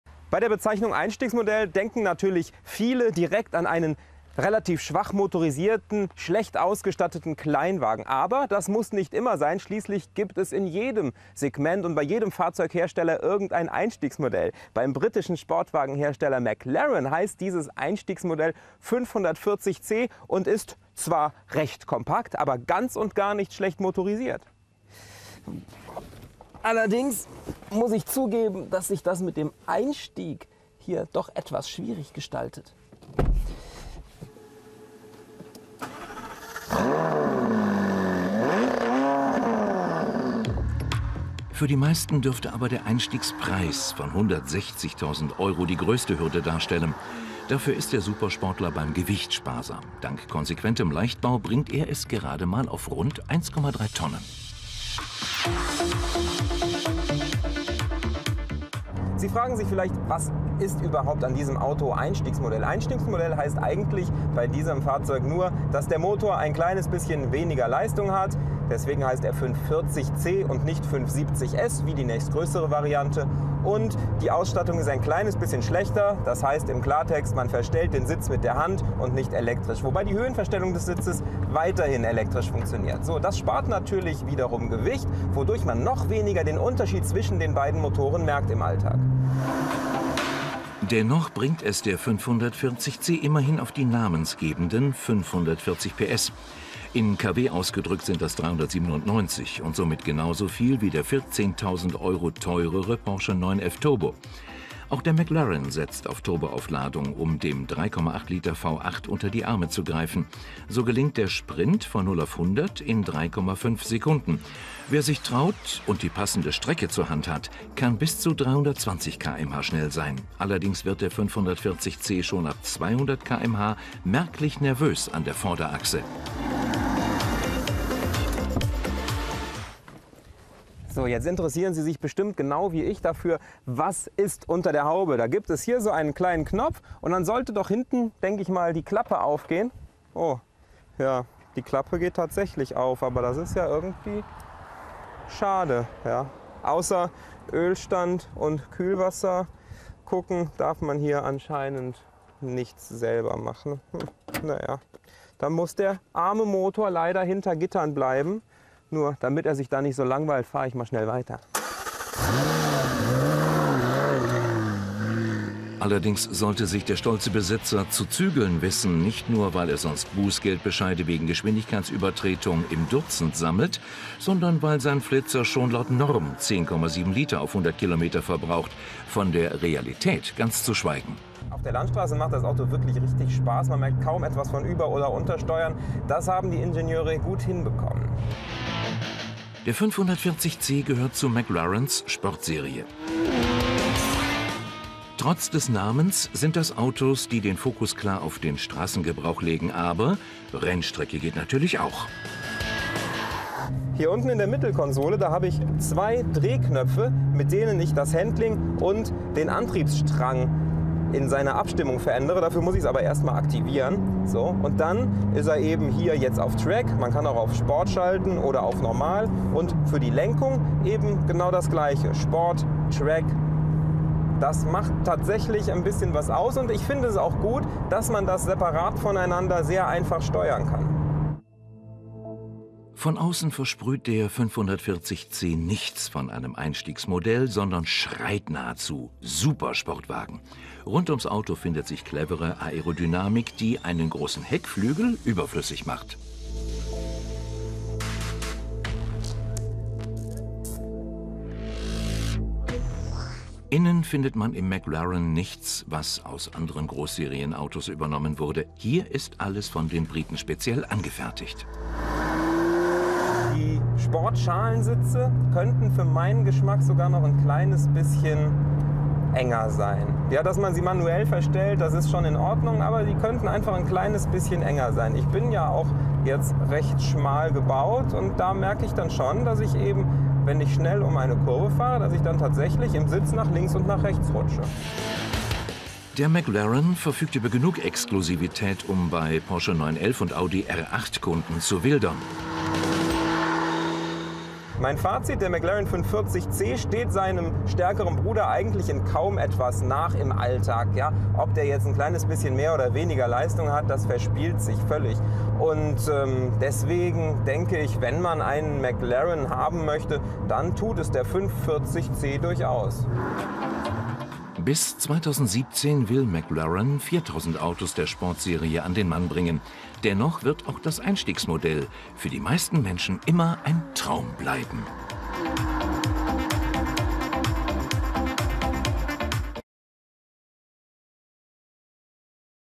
Ein Einstiegsmodell für 160.000 Euro | Motor mobil
Rennwagen für die Straße: Die McLaren-"Sport-Series"-Modelle. Die neue Einstiegsvariante 540C ist nicht nur mit ihren 160.000€ in Deutschland in direkter Schlagdistanz zum Porsche 911 Turbo. 397 KW katapultieren sie in 3,5 Sekunden auf 100 km/h.